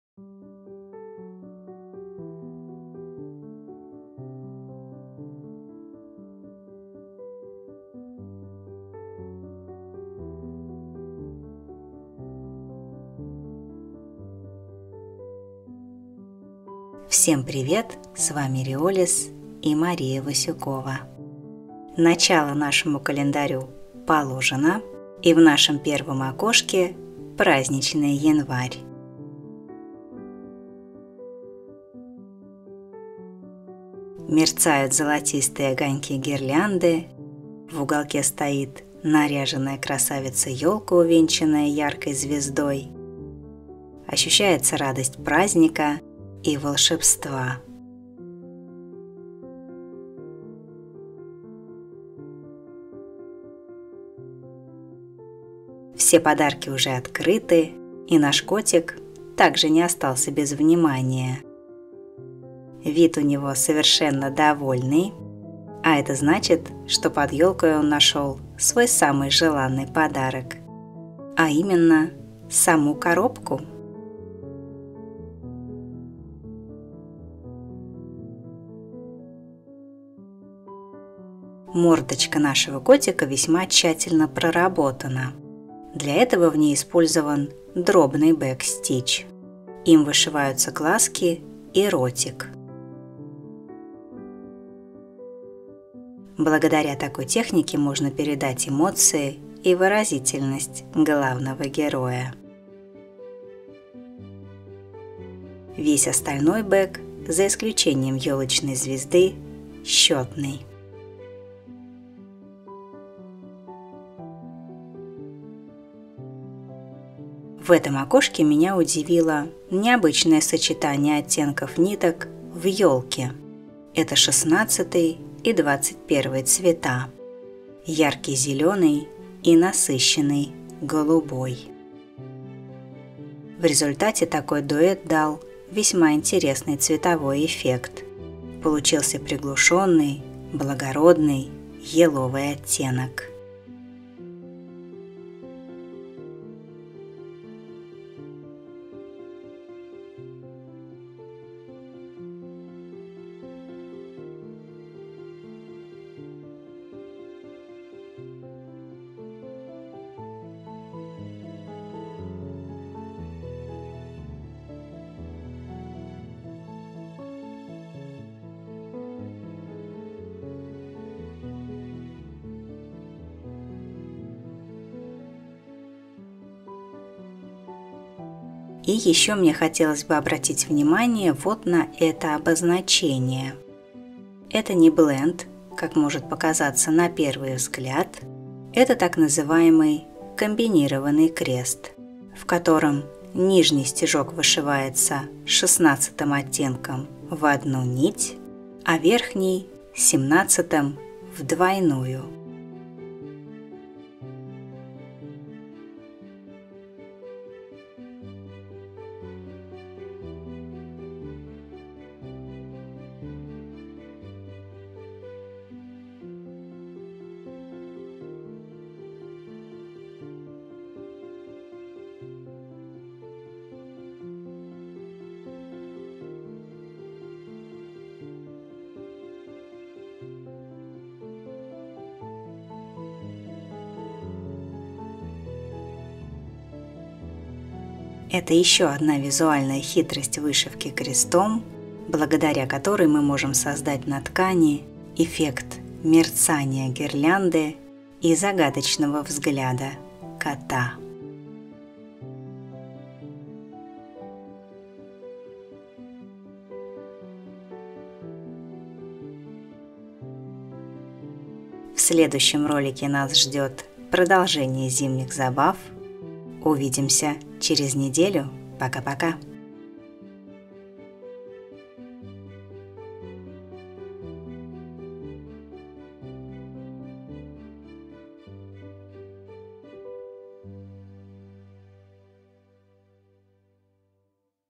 Летний котострой. Часть 2. Январь
Приглашаем вас в новый летний долгострой с отшивом набора "Кошачий календарь".
Каждую неделю мы будем вышивать по одному милейшему котику и делиться с вами результатом.

В этой части мы вышьем январского пушистика и удивимся магии бэка.

Присоединяйтесь к нашему котострою!